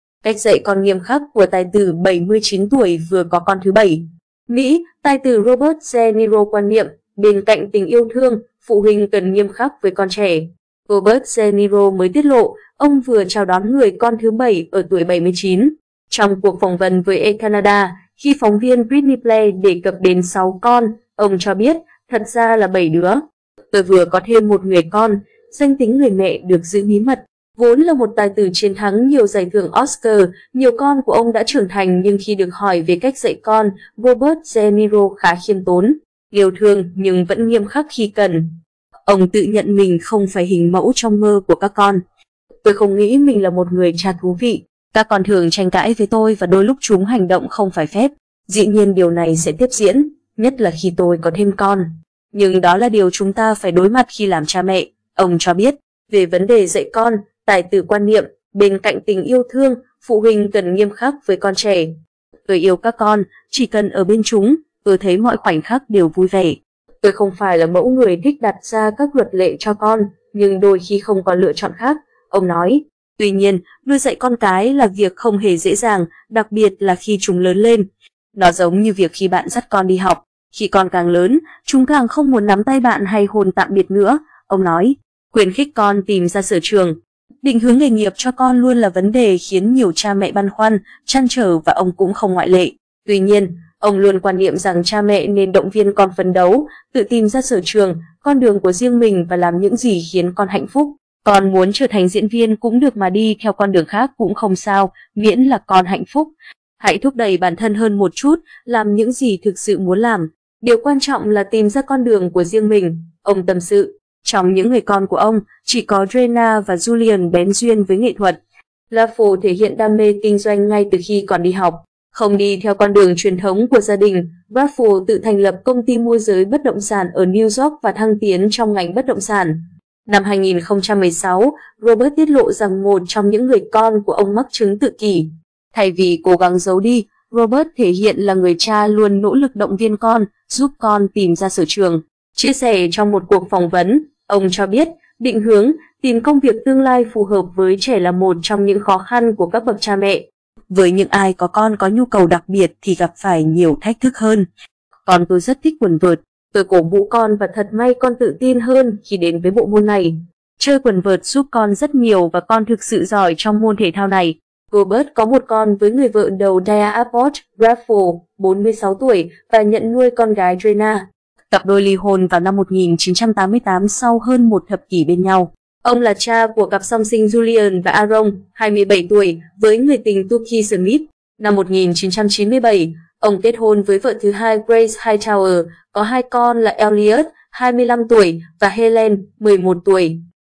Cách dạy con nghiêm khắc của tài tử 79 tuổi vừa có con thứ 7
MỸ - Tài tử Robert De Niro quan niệm, bên cạnh tình yêu thương, phụ huynh cần nghiêm khắc với con trẻ.Ông là cha của cặp song sinh Julian và Aaron (27 tuổi) với người tình Toukie Smith. Năm 1997, ông kết hôn với vợ thứ 2 Grace Hightower, có 2 con là Elliot (25 tuổi) và Helen (11 tuổi).
Tuy nhiên, nuôi dạy con cái là việc không hề dễ dàng, đặc biệt là khi chúng lớn lên. "Nó giống như việc khi bạn dắt con đi học. Khi con càng lớn, chúng càng không muốn nắm tay bạn hay hôn tạm biệt nữa", ông nói.
Robert và con gái Helen trên đường phố.
Yêu thương nhưng vẫn nghiêm khắc khi cần
Khuyến khích con tìm ra sở trường
Vốn là một tài tử chiến thắng nhiều giải thưởng Oscar, con của ông đã trưởng thành nhưng khi được hỏi về cách dạy con, Robert De Niro khá khiếm tốn.
Năm 2016, Robert tiết lộ rằng một trong những người con của ông mắc chứng tự kỷ. Thay vì cố gắng giấu đi, Robert thể hiện là người cha luôn nỗ lực động viên con, giúp con tìm ra sở trường.
Robert De Niro mới tiết lộ, ông vừa chào đón người con thứ 7 ở tuổi 79.
Ông tự nhận mình không phải hình mẫu trong mơ của các con. "Tôi không nghĩ mình là một người cha thú vị. Các con thường tranh cãi với tôi và đôi lúc chúng hành động không phải phép. Dĩ nhiên điều này sẽ tiếp diễn, nhất là khi tôi có thêm con. Nhưng đó là điều chúng ta phải đối mặt khi làm cha mẹ", ông cho biết.
Chia sẻ trong một cuộc phỏng vấn, ông cho biết: "Định hướng, tìm công việc tương lai phù hợp với trẻ là một trong những khó khăn của các bậc cha mẹ. Với những ai có con có nhu cầu đặc biệt thì gặp phải nhiều thách thức hơn. Con tôi rất thích quần vợt. Tôi cổ vũ con và thật may con tự tin hơn khi đến với bộ môn này. Chơi quần vợt giúp con rất nhiều và con thực sự giỏi trong môn thể thao này".
Trong cuộc phỏng vấn với ET Canada, khi phóng viên Brittnee Blair đề cập đến 6 con, ông cho biết: "Thật ra là 7 đứa. Tôi vừa có thêm 1 người con". Danh tính người mẹ được giữ bí mật.
Robert và người con Elliot
Trong những người con của ông, chỉ có Dreana và Julian bén duyên với nghệ thuật. Raphel thể hiện đam mê kinh doanh ngay từ khi còn đi học. Không đi theo con đường truyền thống của gia đình, Raphel tự thành lập công ty môi giới bất động sản ở New York và thăng tiến trong ngành bất động sản.
"Con muốn trở thành diễn viên cũng được mà đi theo con đường khác cũng không sao, miễn là con hạnh phúc. Hãy thúc đẩy bản thân hơn một chút, làm những gì thực sự muốn làm. Điều quan trọng là tìm ra con đường của riêng mình", ông tâm sự.
Robert có 1 con với người vợ đầu Diahnne Abbot, Raphel (46 tuổi) và nhận nuôi con gái Dreana. Cặp đôi ly hôn vào năm 1988 sau hơn một thập kỷ bên nhau.
Định hướng nghề nghiệp cho con luôn là vấn đề khiến nhiều cha mẹ băn khoăn, trăn trở và ông cũng không ngoại lệ. Tuy nhiên, ông luôn quan niệm rằng cha mẹ nên động viên con phấn đấu, tự tìm ra sở trường, con đường của riêng mình và làm những gì khiến con hạnh phúc.
Về vấn đề dạy con, tài tử quan niệm, bên cạnh tình yêu thương, phụ huynh cần nghiêm khắc với con trẻ. "Tôi yêu các con, chỉ cần ở bên chúng, tôi thấy mọi khoảnh khắc đều vui vẻ. Tôi không phải là mẫu người thích đặt ra các luật lệ cho con, nhưng đôi khi không còn lựa chọn khác", ông nói.